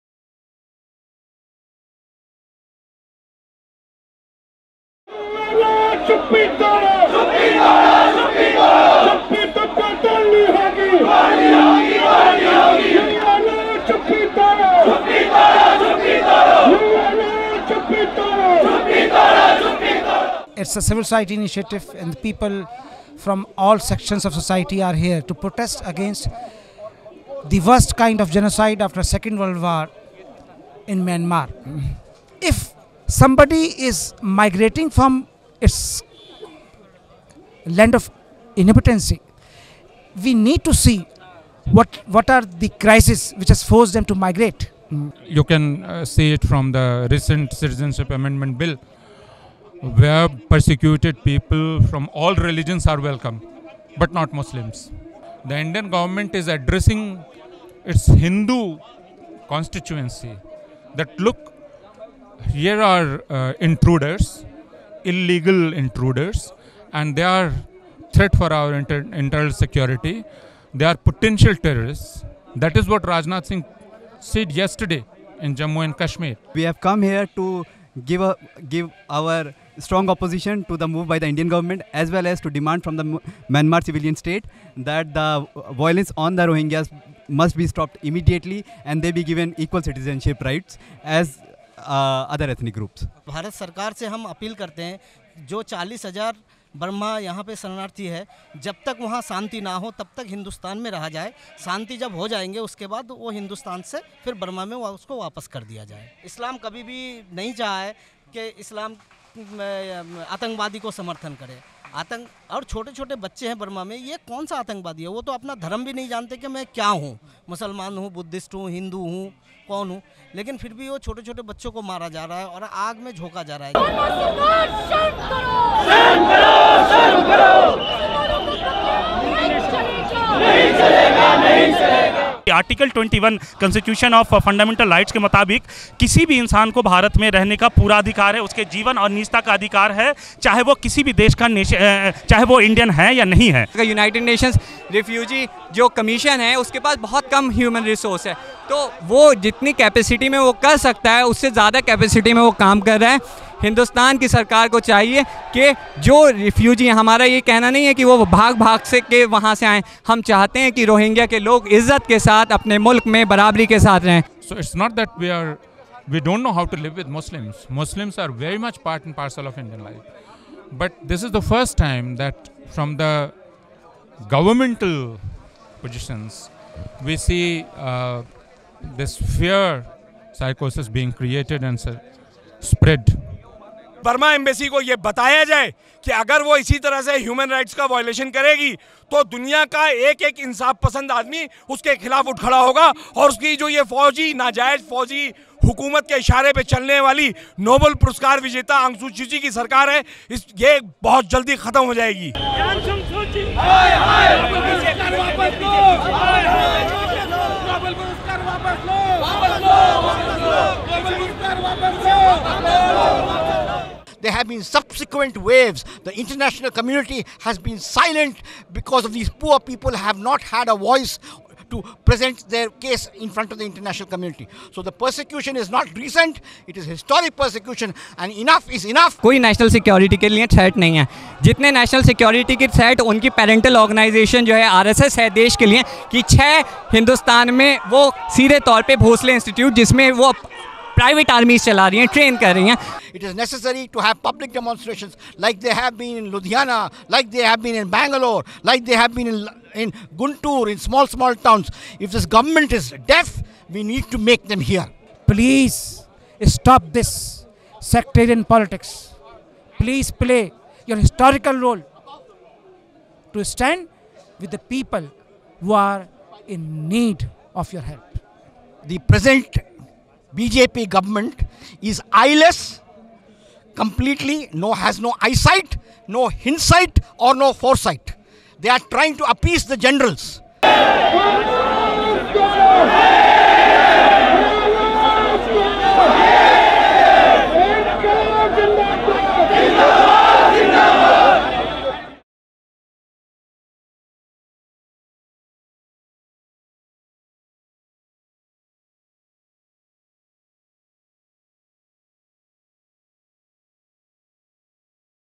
Thousands Gather to Protest Deportation of Rohingya Refugees
On 13th September 2017, people gathered in thousands at the Myanmar Embassy in Delhi to protest the proposed deportation of Rohingya refugees and the abysmal treatment meted out to them in their own country.